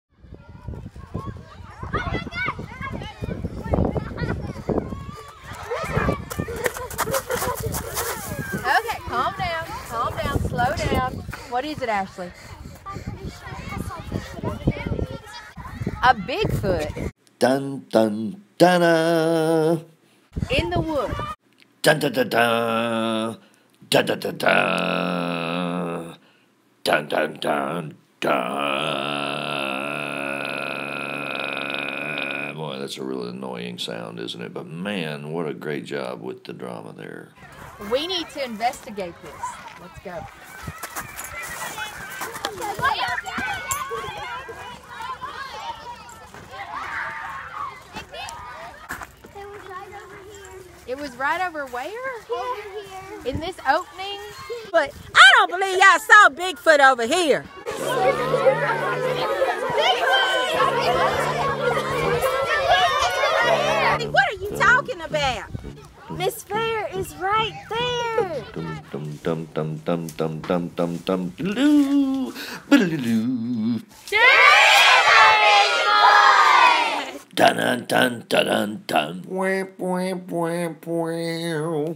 This Could be the Only REAL Big Foot Footage In Focus!
These kids really do see a BIG FOOT!

Happy Halloween!